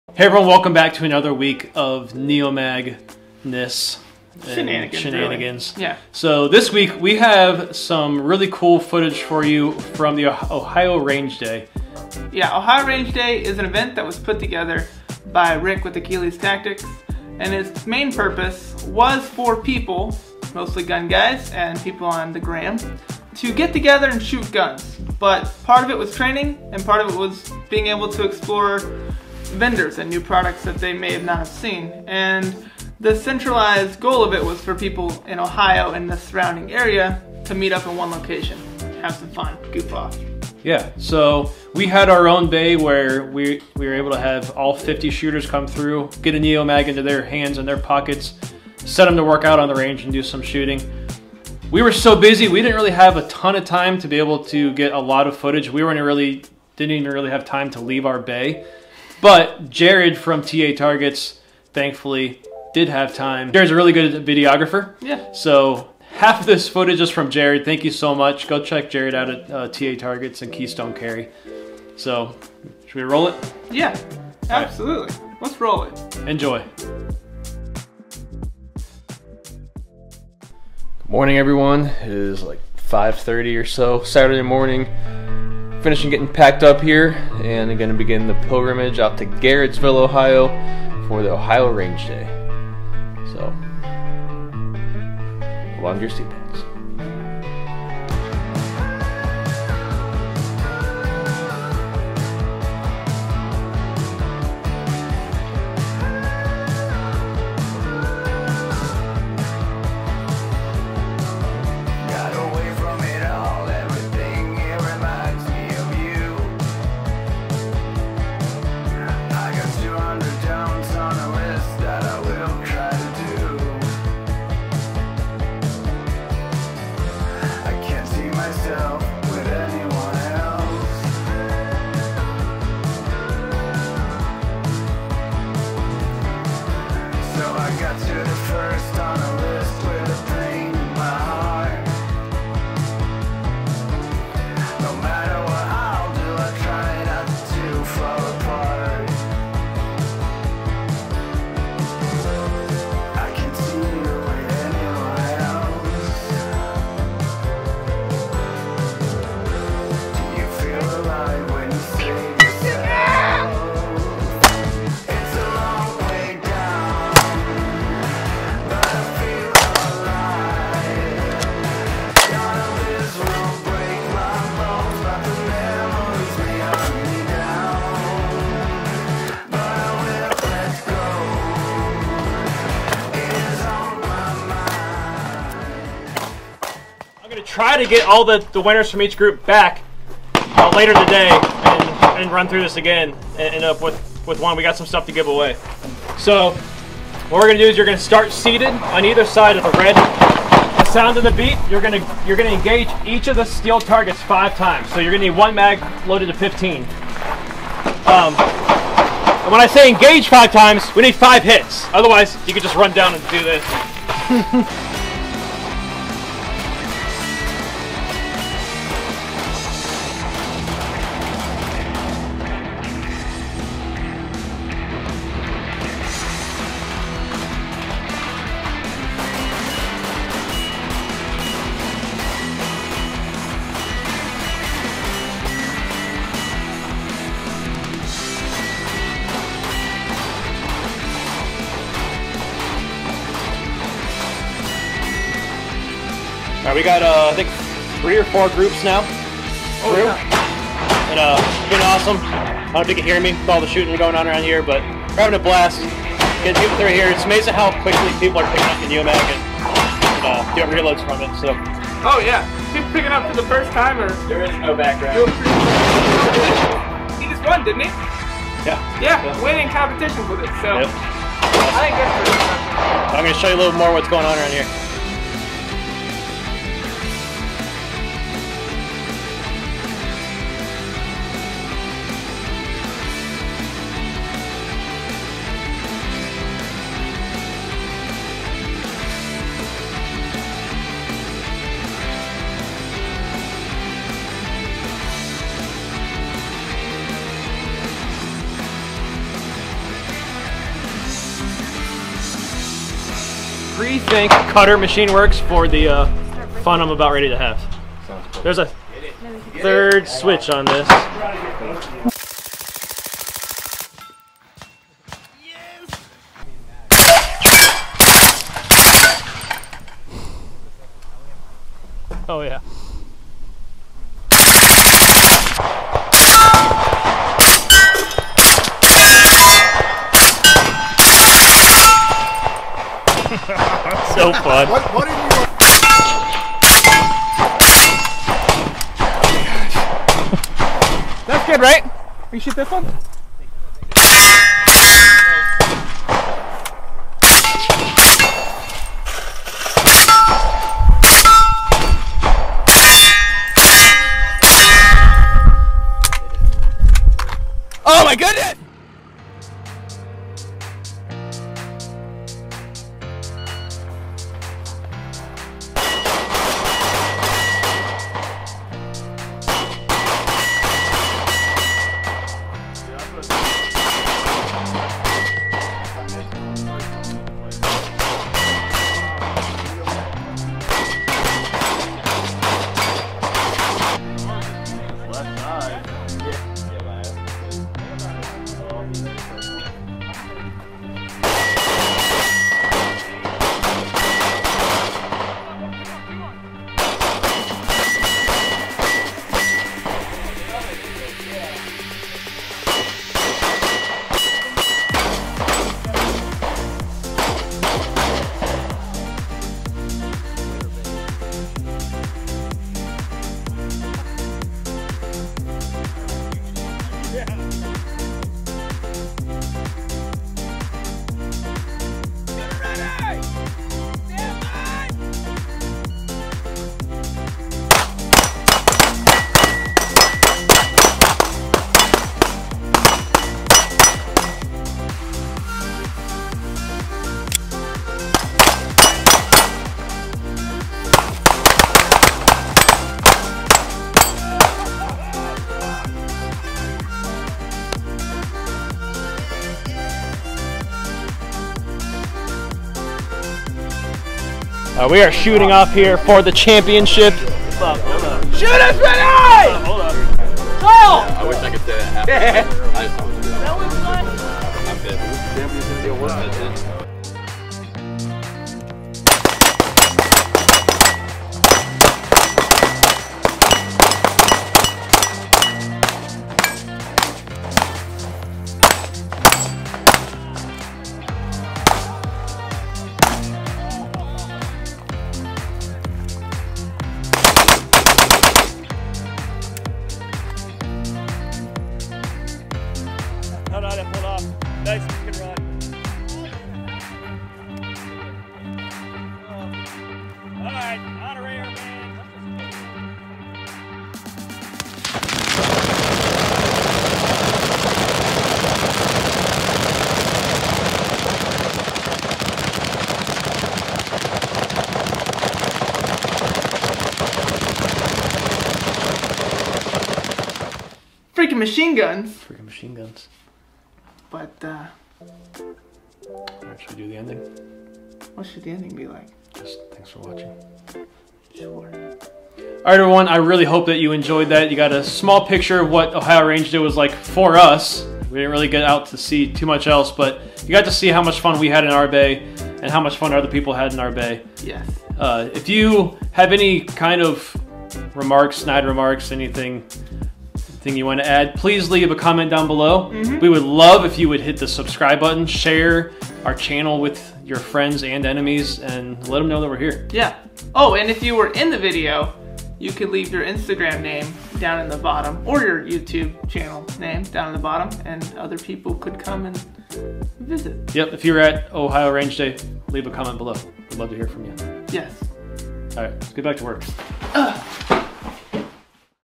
GREATEST RANGE DAY EVER!? OHIO RANGE DAY 2019
This week we give you a glimpse into our bay at the Ohio Range Day 2019.  We had 50 shooters come through and experience working with the NeoMag.  But more importantly was the opportunity for us to meet so many great people.

If you are interested in coming to ORD 2020 keep an eye out for more information soon.  ORD 2020 is going to be much bigger and awesomer.

Big thanks to Jared Daub from TA Targets and @keystonecarry for some awesome footage to roll in with ours.  Thanks to Rick and his team from Achilles Tactics for organizing the event.  And lastly to the guys from Digital Alchemy for the amazing photos and video.